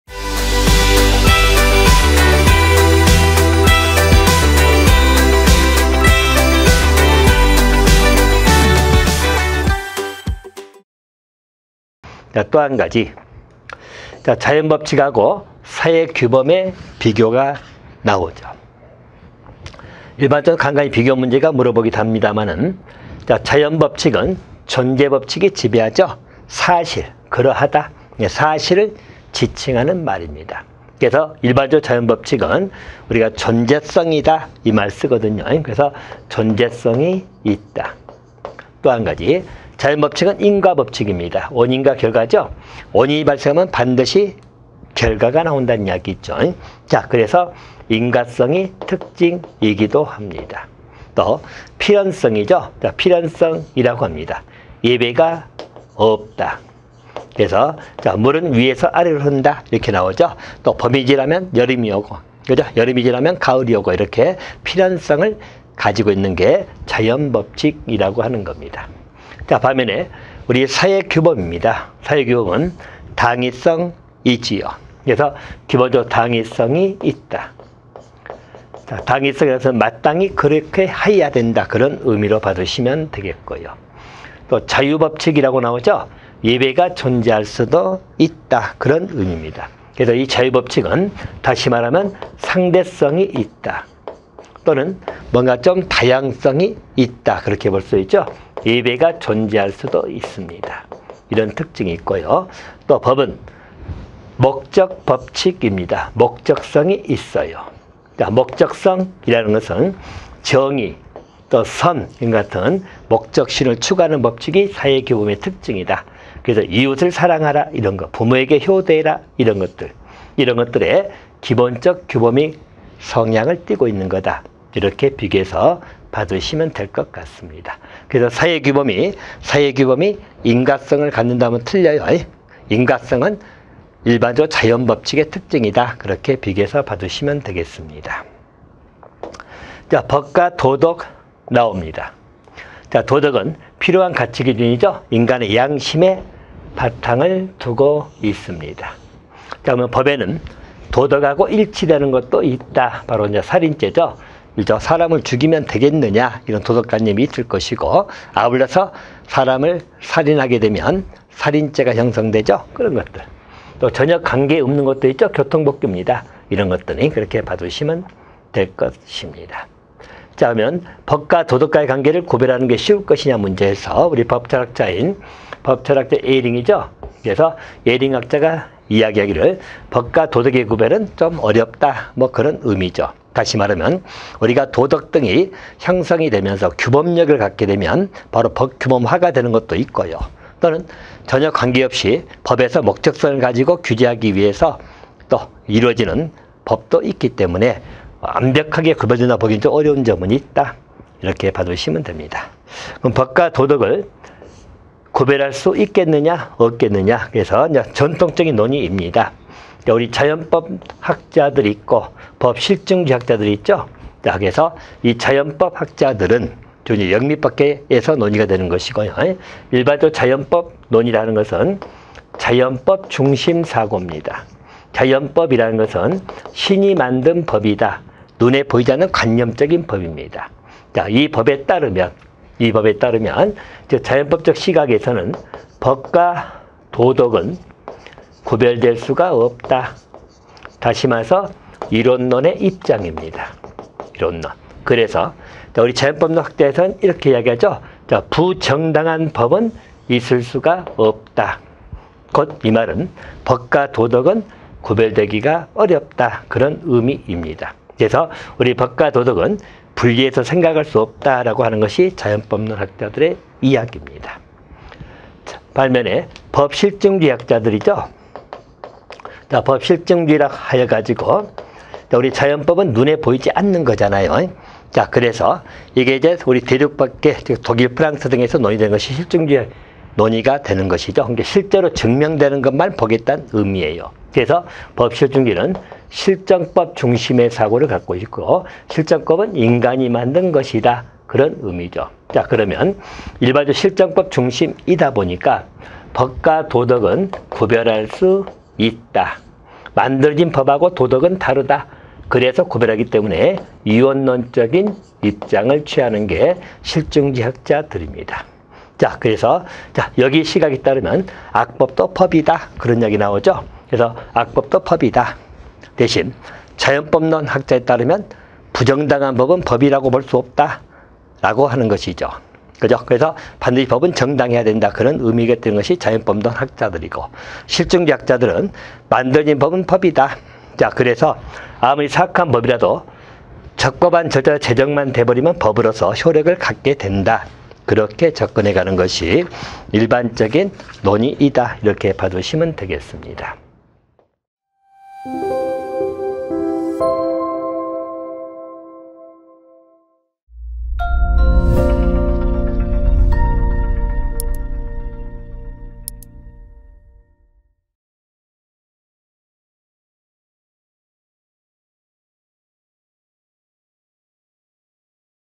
2024 경비지도사 법학개론 | 이영표쌤의 법학 일반 part.2 [에듀마켓]
안녕하세요, 에듀마켓입니다~ 🙋🏻‍♂

2024 경비지도사 최강 교수진!
베테랑 이영표 교수님의 법학개론 고득점 비법 👨🏻‍🏫✅

"법학 일반" 샘플강의입니다.

에듀마켓 자격증 과정에 관해 보다 자세히 알고 싶으신 분은
아래 링크를 통해 알아보실 수 있습니다.👇👇👩🏻‍💻

감사합니다!🙇🏻‍♂